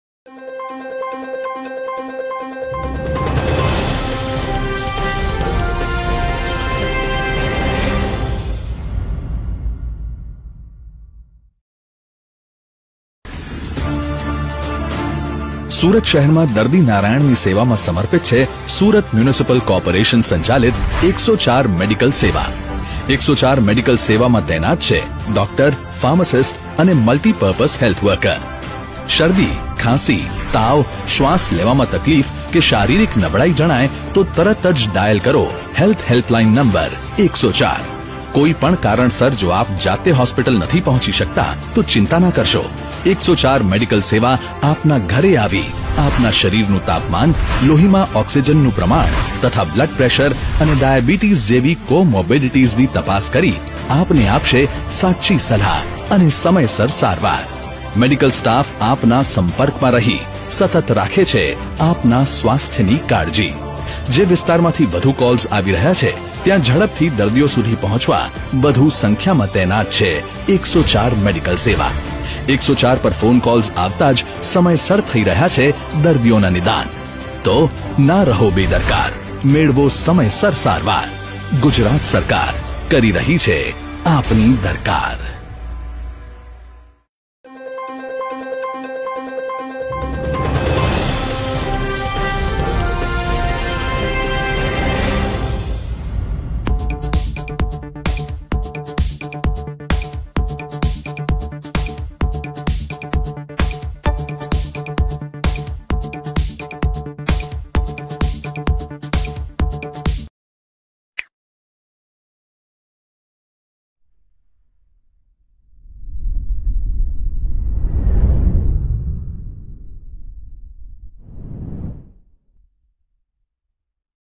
COVID19 MEDICAL SEVA SURAT BY SMC
કોવિડના સમયમાં દર્દીઓની સેવા માટે અથાક પરિશ્રમ કરે છે સુરત મહાનગરપાલિકા સંચાલિત ૧૦૪ મેડિકલ સેવા. શરદી, ખાંસી, શ્વાસ લેવામાં તકલીફ અને શારીરિક નબળાઈ જણાય તો ડાયલ કરો ૧૦૪ હેલ્પ હેલ્થલાઈન નંબર.